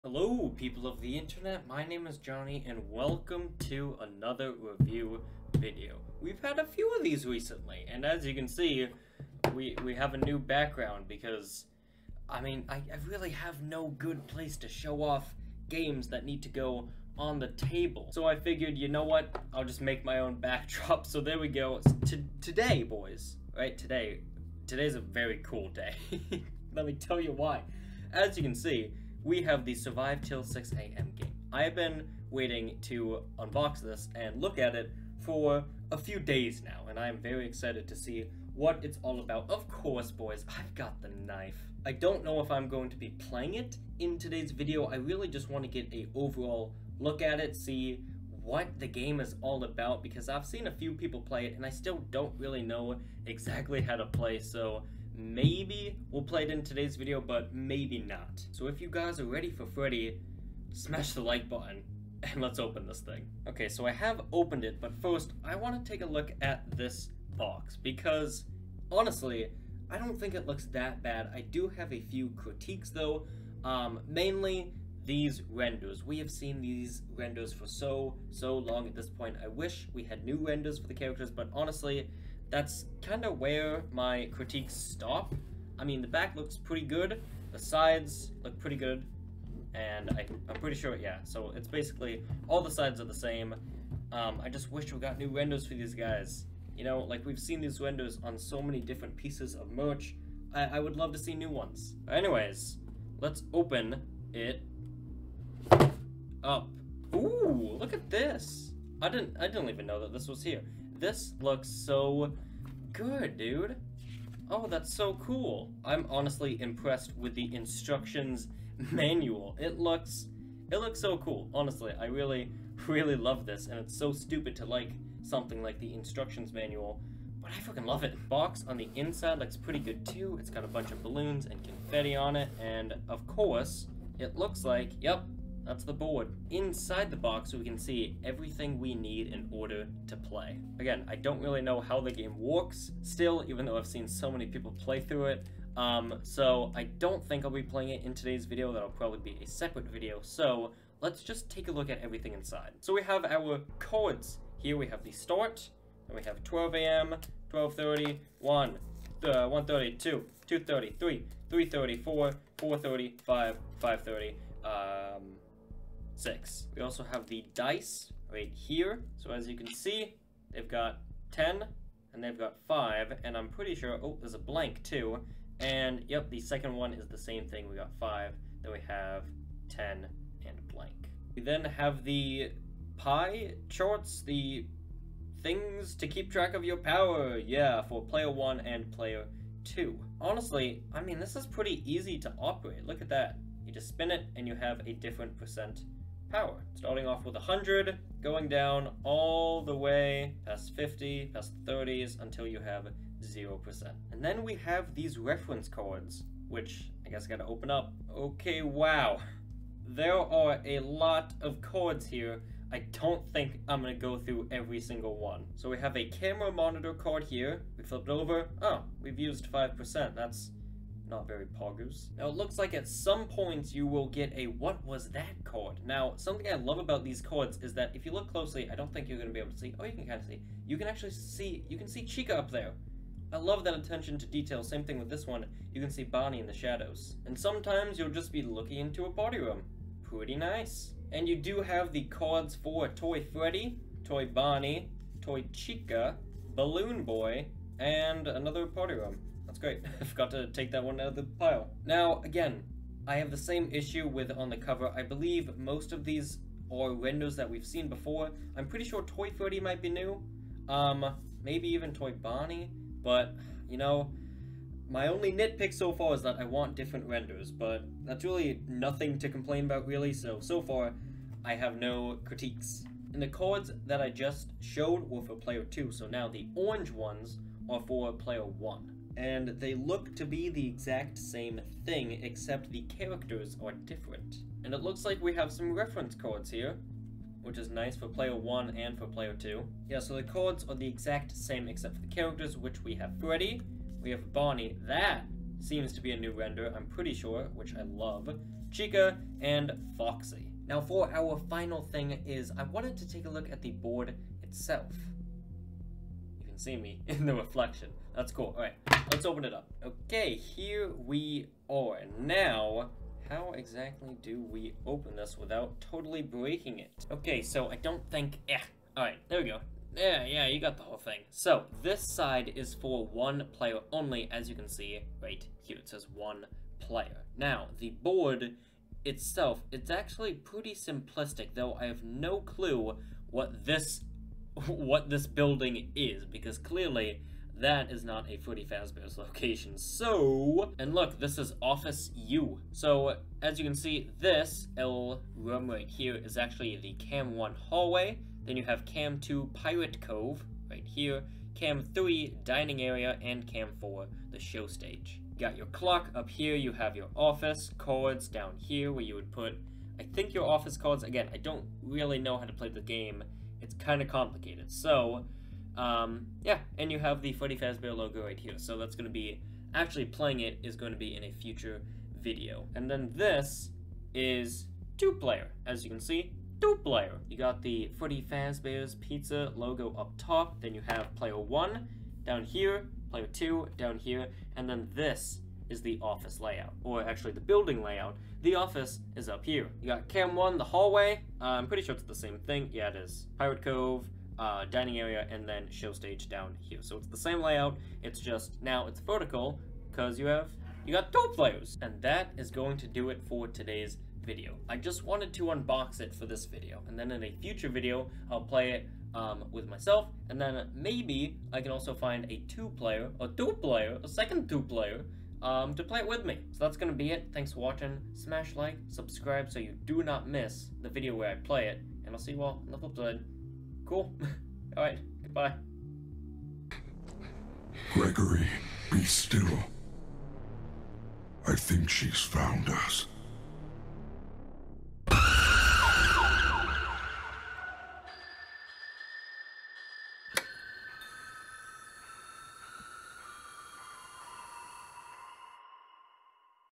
UNBOXING FUNKO'S BRAND NEW "FNAF SURVIVE 'TIL 6AM GAME!!"
Unboxing and reviewing Funko's FIVE NIGHTS AT FREDDY'S SURVIVE 'TIL 6AM GAME!!